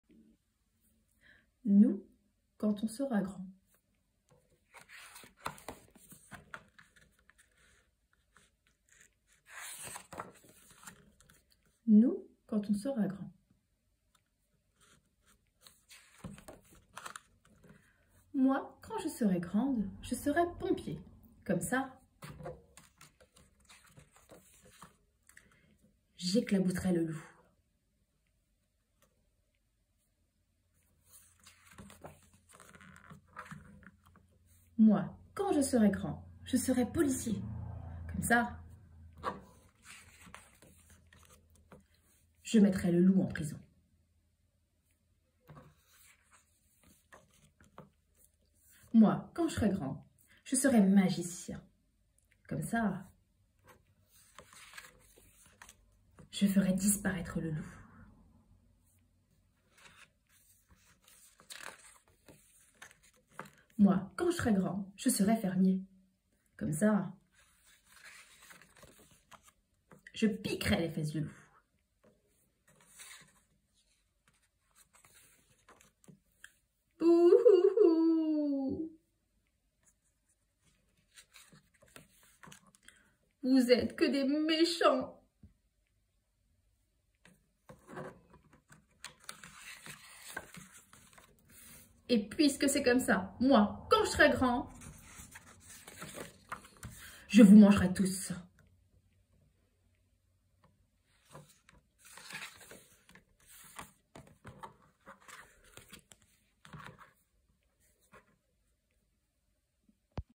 Nous, quand on sera grands (Matthieu Maudet, Jean Leroy)
Thème : attention aux a priori )))
Retrouvez toutes nos lectures (gratuites) d'album jeunesse sur notre blog Raconte-moi!